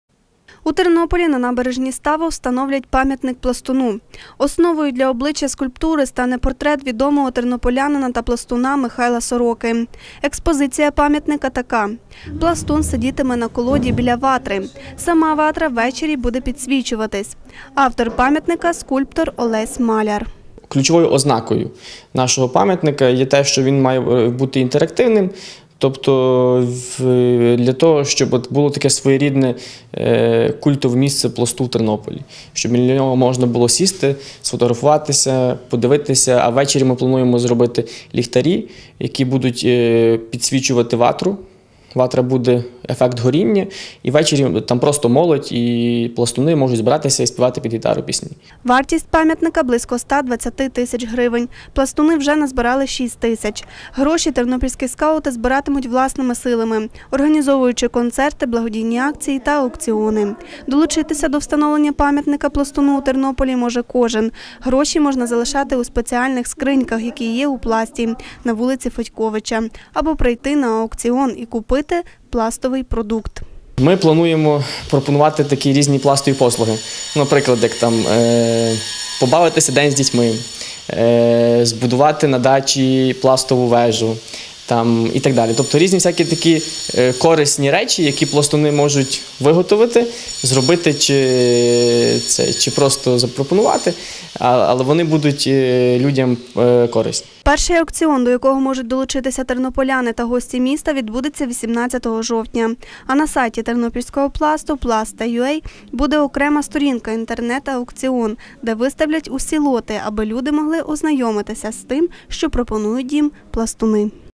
У Тернополі буде пам'ятник Пласту
У Тернополі буде пам'ятник Пласту. Аби виготовити монумент треба 120 тисяч. Пластуни зібрали лише 6 тисяч гривень. Тернопільські скаути проводитимуть благодійні акції, концерти, ярмарки для тернополян та гостей міста аби зібрати решту суми. Перший аукціон запланували на 18 жовтня.